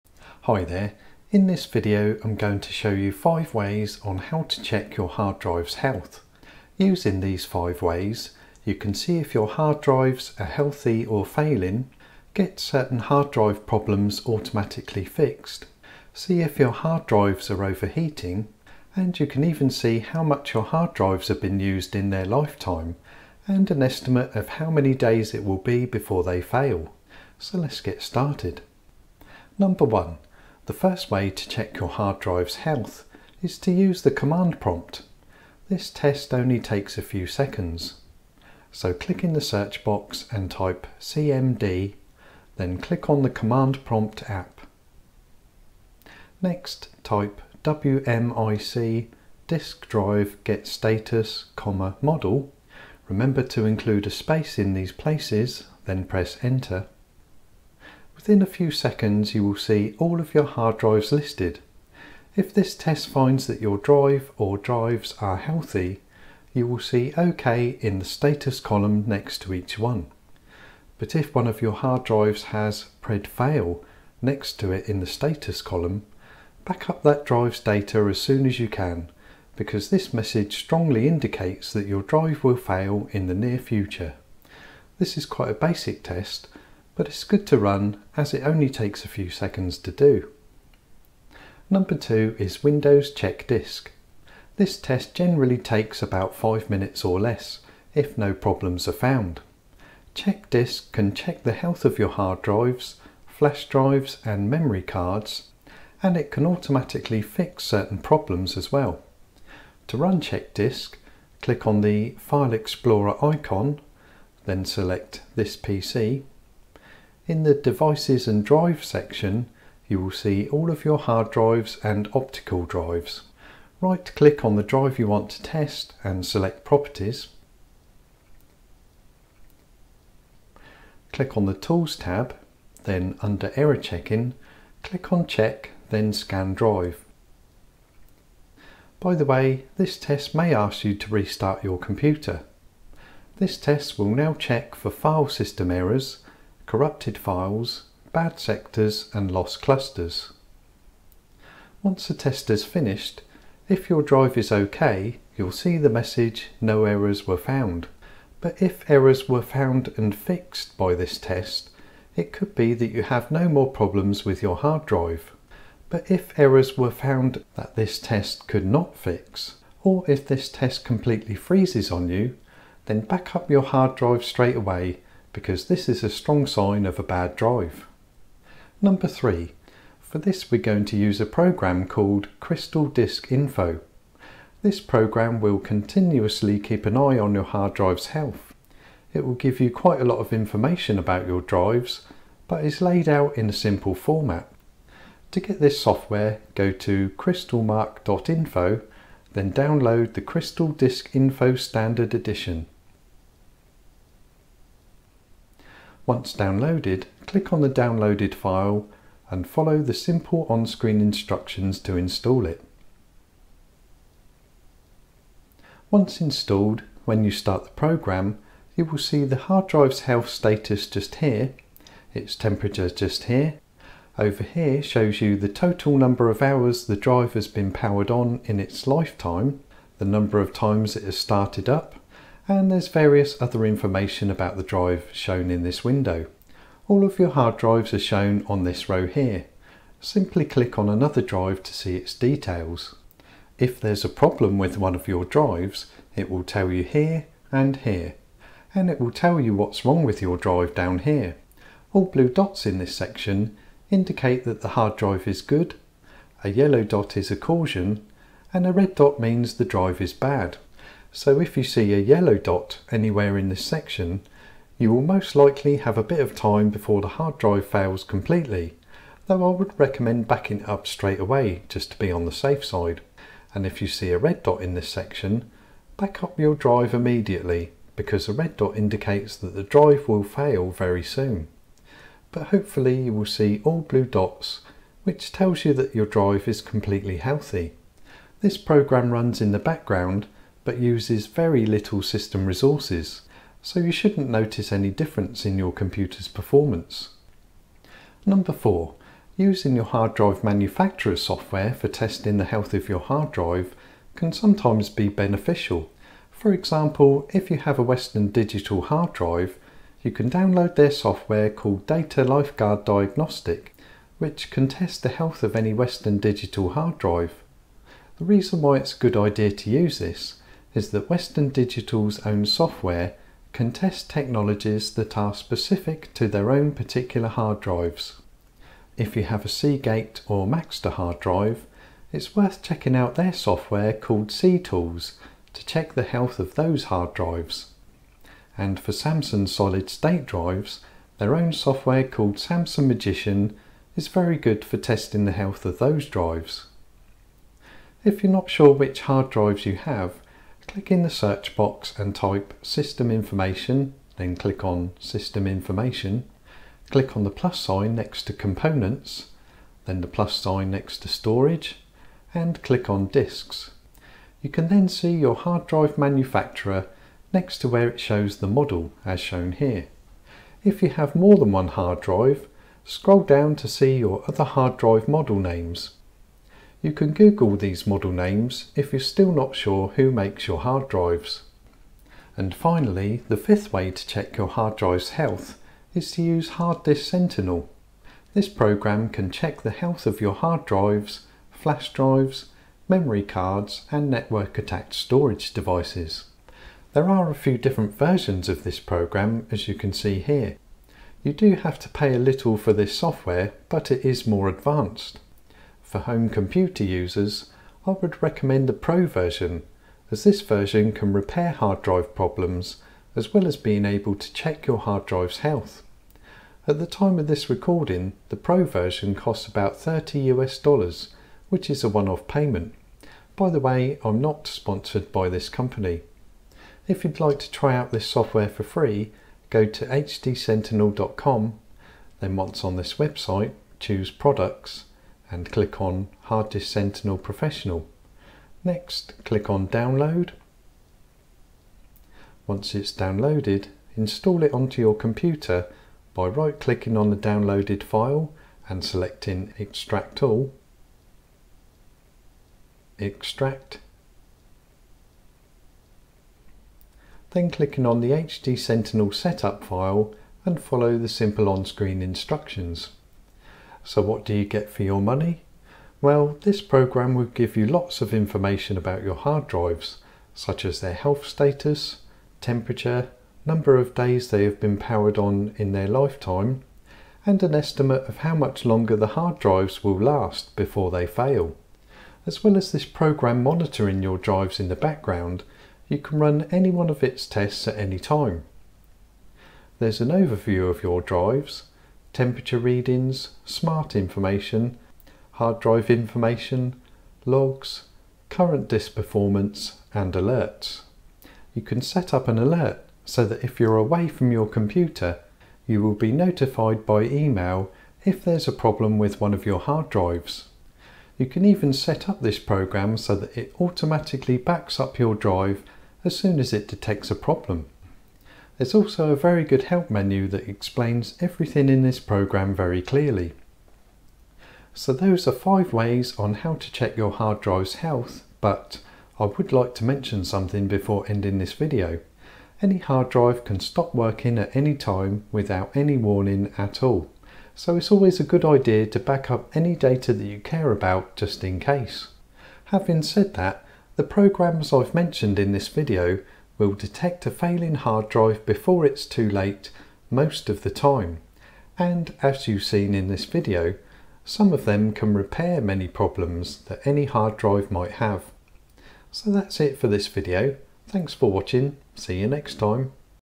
5 Ways To Check Your Hard Drive's Health - Windows 10
See an estimate of how much longer it will be before your hard drives fail, how many hours your hard drive has been used in its lifetime, get some of your hard drive problems fixed automatically (if any), see how hot your hard drives are running and if they are overheating.
It's always good to check your hard drive's health just in case there's a problem that you don't know about. These ways of checking your hard drive's health are for solid state drives and hard disk drives. You can even check the health of a NAS (Network Attached Storage) device.

Command for the first hard drive check...
wmic diskdrive get status, model